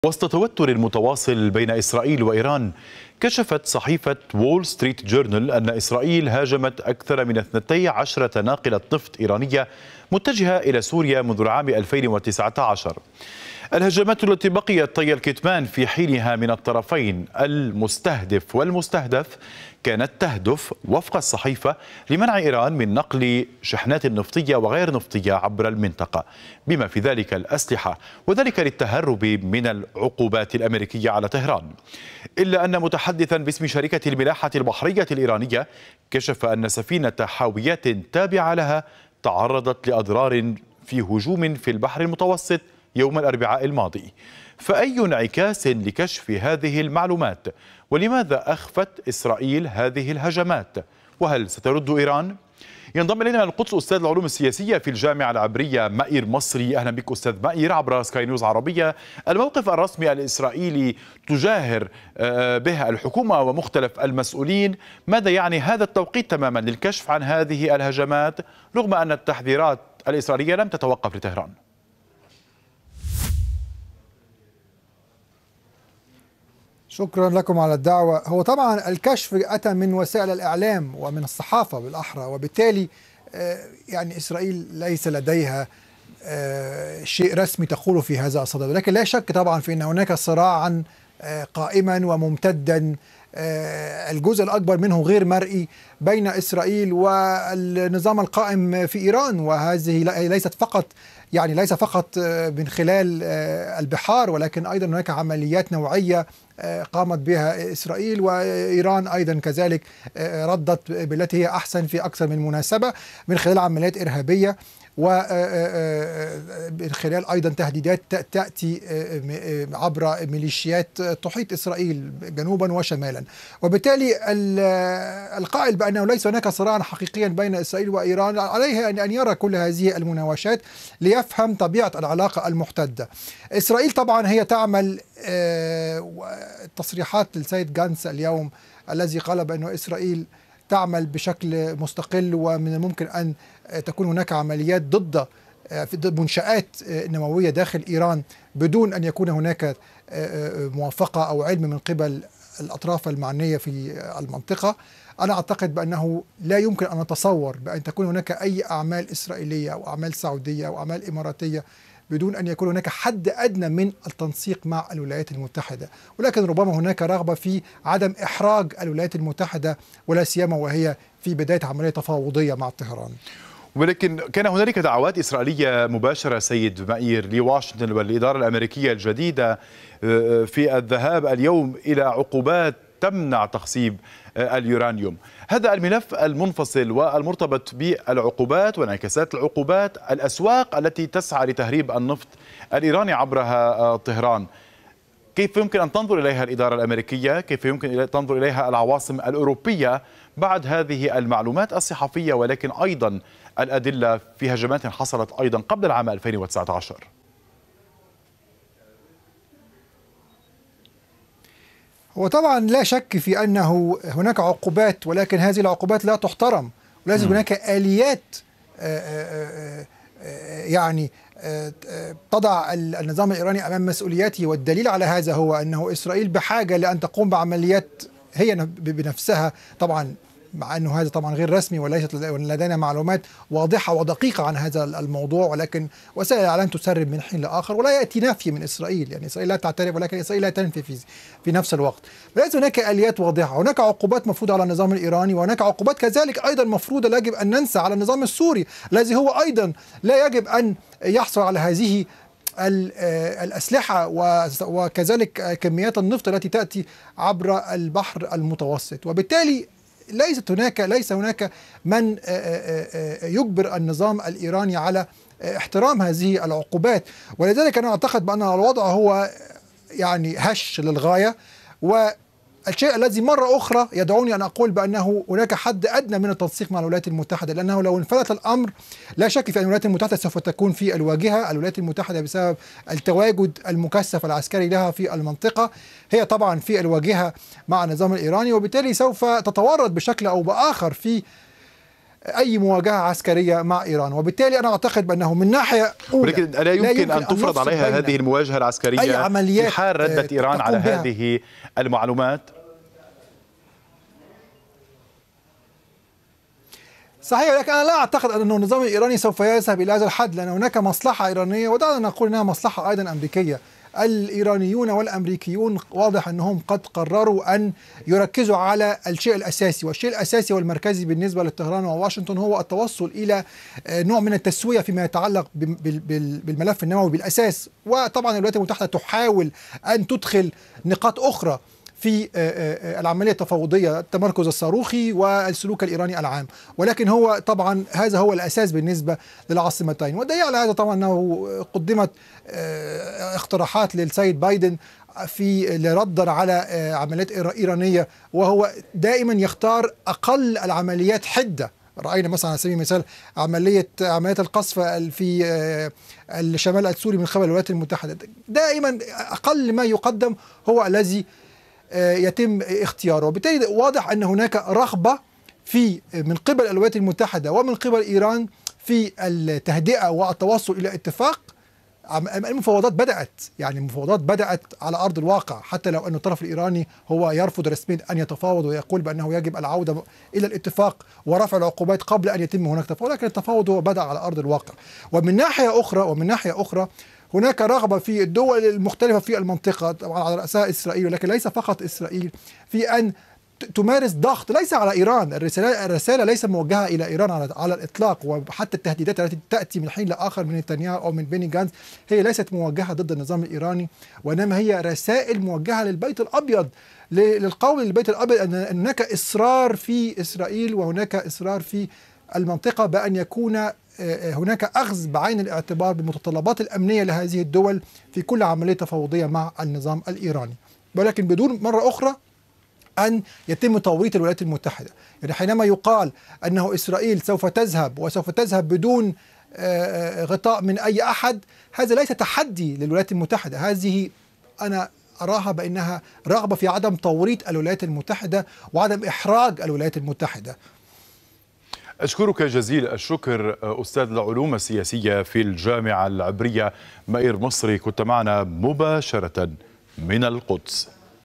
سفن إيرانية في مرمى الاستهداف الإسرائيلي منذ 2019
وسط توتر متواصل بين إسرائيل وإيران، كشفت صحيفة "وول ستريت جورنال" أن إسرائيل هاجمت أكثر من اثنتي عشرة ناقلة نَفط إيرانية متجهةً إلى سوريا منذ العام ألفين وتسعة عشر.
الهجمات التي بقيت طي الكتمان في حينها من الطرفين المستهدِف والمستهدَف، كانت تهدف بحسب الصحيفة لمنع إيران من نقل شِحنات نفطية وغير نفطية عبر المنطقة، بما في ذلك الأسلحةُ، للتهرب من العقوبات الأميركية.
إلاّ أن متحدثاً باسم شركة الملاحة البحرية الإيرانية كشف أن سفينة حاويات تابعةً لها تعرضت لأضرار في هجوم بالبحرالمتوسط يوم الأربعاء الماضي.
فأي انعكاس لكشف هذه المعلومات؟ ولماذا أخفت إسرائيل هذه الهجمات؟ وهل سترد إيران؟